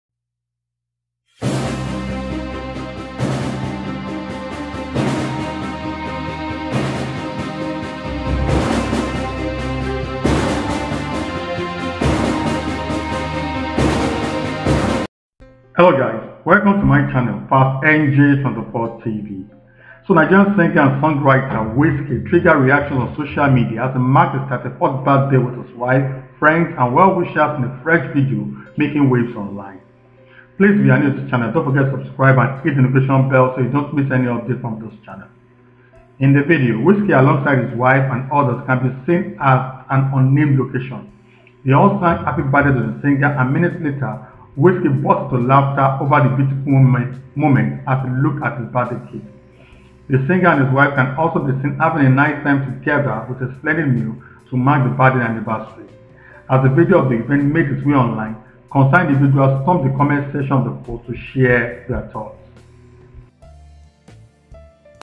WizKidAt34: Wizkid celebrating 34th birthday with wife, friends
Nigerian singer and songwriter Wizkid triggered reactions on social media as he marked his 34th birthday with his wife, friends, and well-wishers in a fresh video making waves online.